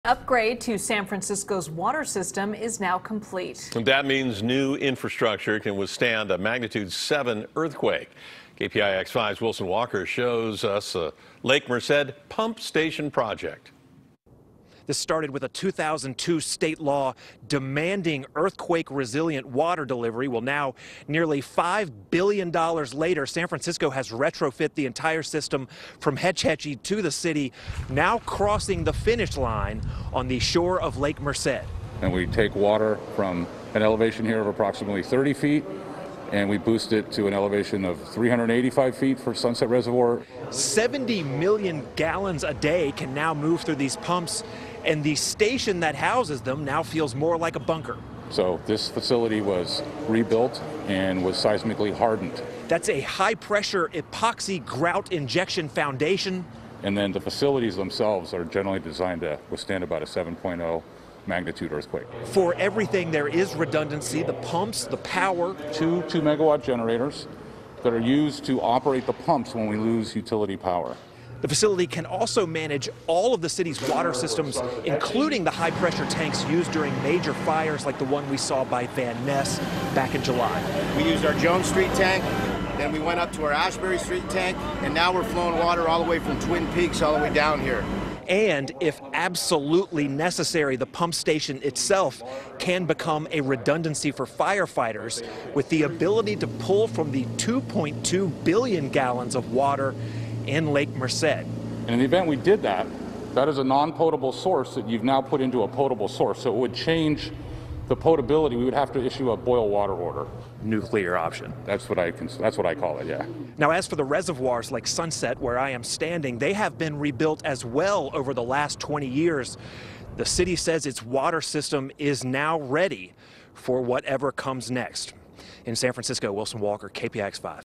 Earthquake Retrofit Work Complete at Lake Merced Pump Station
After spending $5 billion San Francisco has retrofit the water system from Hetch Hetchy to the city, just now crossing the finish line on the shore of Lake Merced. Wilson Walker reports. (10-16-20)